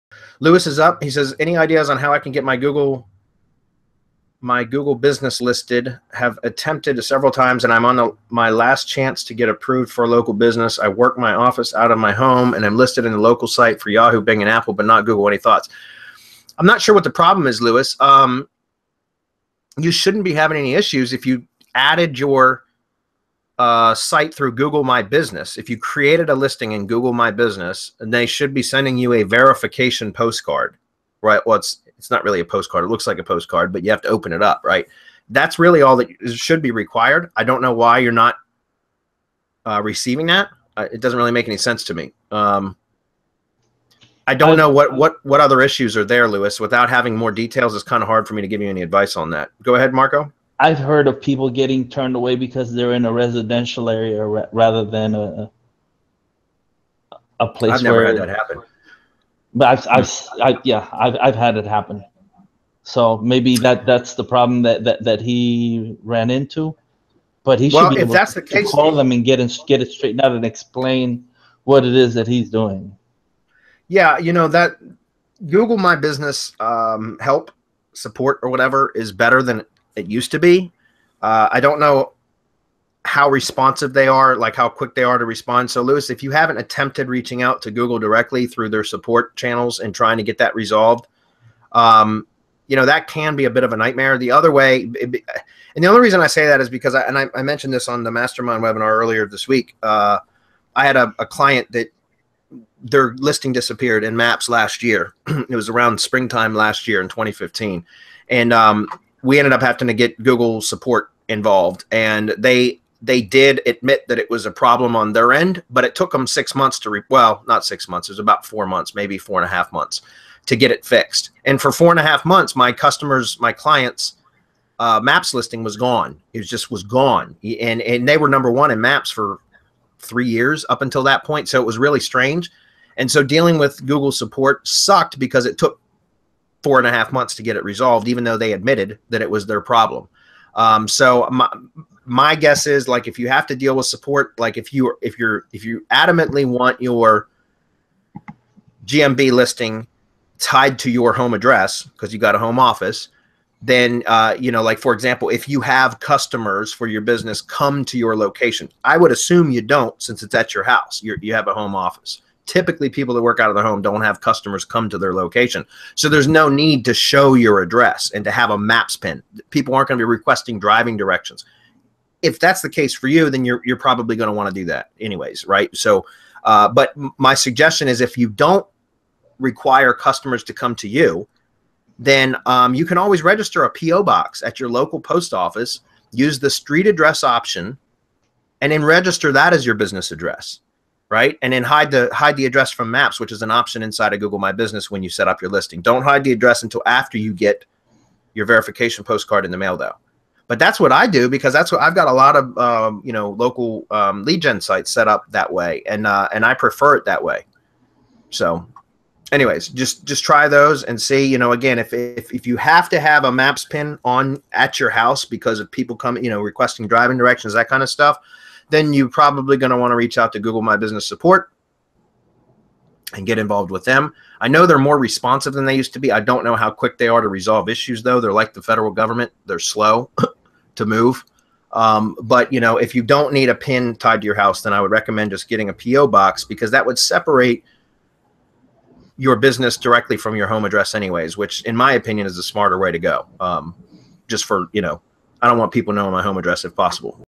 How To Get Your Local Business Listed On Google?
In episode 111 of Semantic Mastery's weekly Hump Day Hangouts one participant asked for recommendations on how to get your business listed in Google.

The exact question was:

any ideas on how I can get my google business listed have attempted several times and I am on my last chance to get approved for a local business I work my office out of my home and am listed in all the local site for yahoo,bing and apple but not google any thoughts